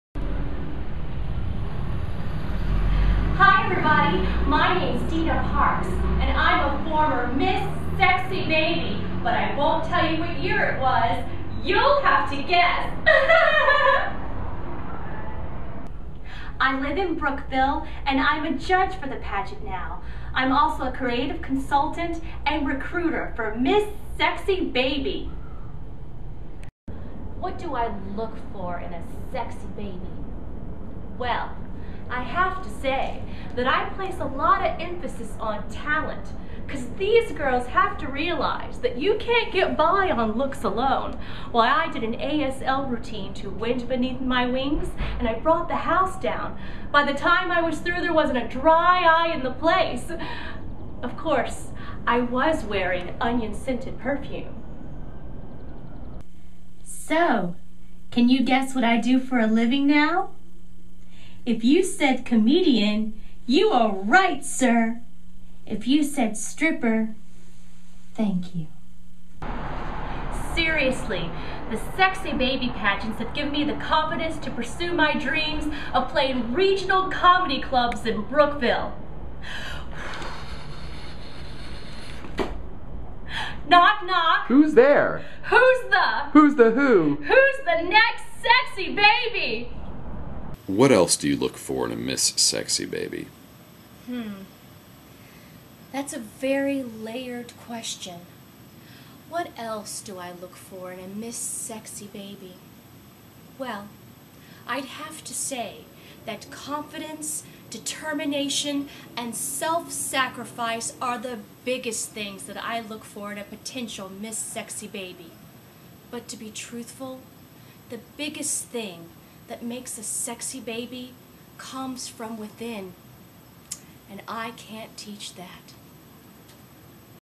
Miss Sexy Baby testimonial- Dina Parks
Ms. Dina Parks share with us what it takes to be crowned Miss Sexy Baby! See Hell in a Handbag's Sexy Baby, a child beauty pageant musical for adults now playing at Mary's Attic!